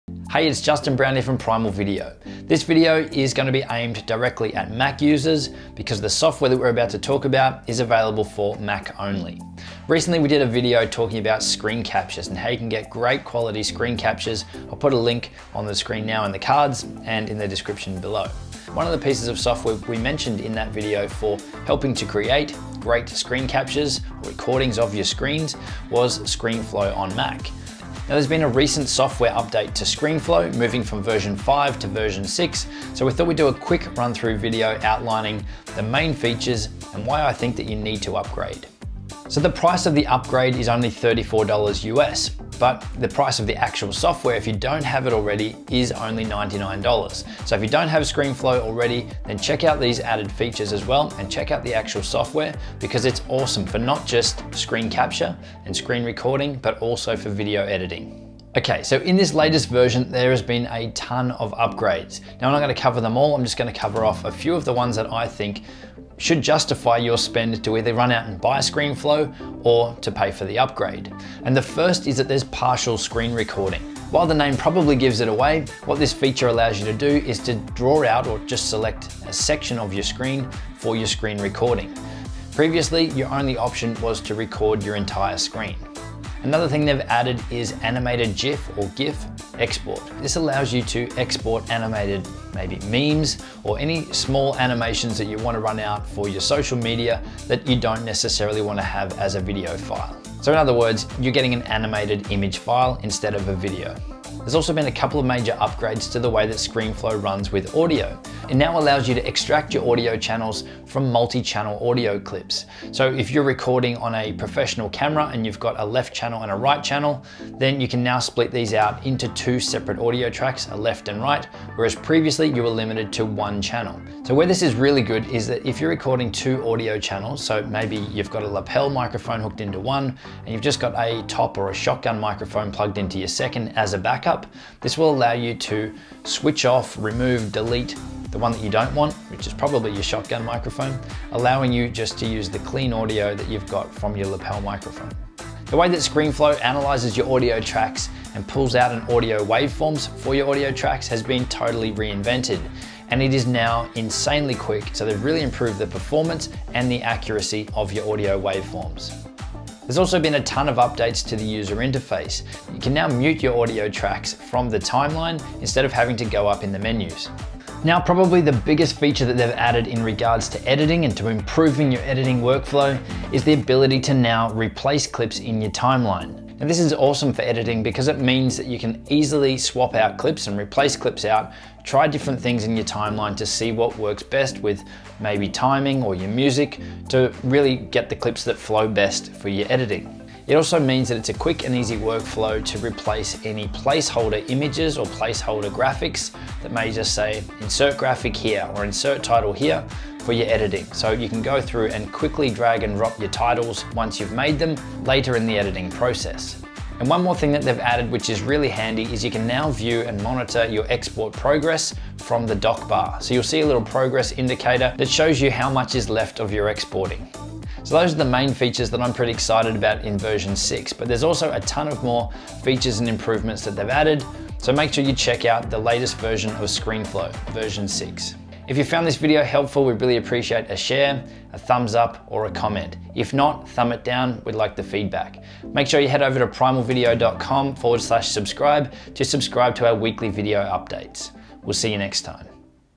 Screenflow 6: What’s new? and should you upgrade from Screenflow 5?
If you're using ScreenFlow (or looking to make the leap), version 6 recently came out. The question is, should you spend the money to update? Here are my thoughts...

A LOT of entrepreneurs in the Primal Video community are using Screenflow for their editing, and that’s no surprise. It’s a great piece of software for an awesome price tag (I use it often myself!)

Screenflow was recently updated to version 6, packing in some pretty cool new additions and tweaks. If you’re running Mac and are using Screenflow (or considering making the leap), the question is, should you spend the money to update?

This video runs through my thoughts on whether you should upgrade, and why!

If you found this video helpful, we'd really appreciate a share, thumbsup or a comment. to receive all our weekly updates!

Related Content